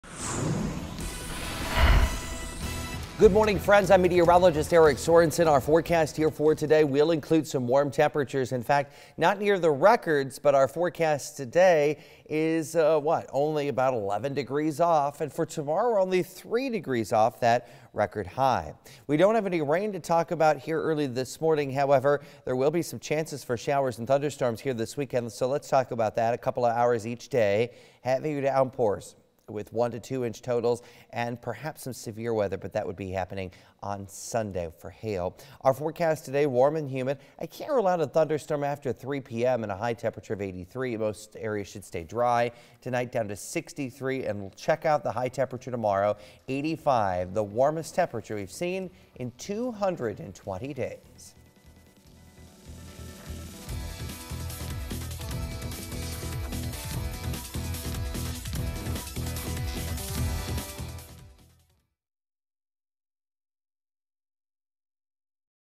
StormTrack 8 Morning Forecast May 6, 2015
Thick fog will burn off between 7 and 10 this morning. We'll have a partly cloudy sky above with mainly dry conditions. Some thunderstorms will be possible west of the Quad Cities during the mid-afternoon. A few of these could move northeast into the metro around dinnertime. They should be weakening and pretty isolated as they approach. Temperatures will surge into the 80s, making this the warmest day so far in 2015.

We'll be pretty quiet overnight with warm and humid conditions. We'll drop to 63 under a partly cloudy sky.

Most of Thursday will be dry, warm, and humid. Temperatures will surge into the middle 80s with a strong south wind.

Thunderstorms will become more numerous Thursday night into Friday. Scattered showers and thunderstorms will be around this weekend with some heavy downpours. Severe weather will be possible on Sunday.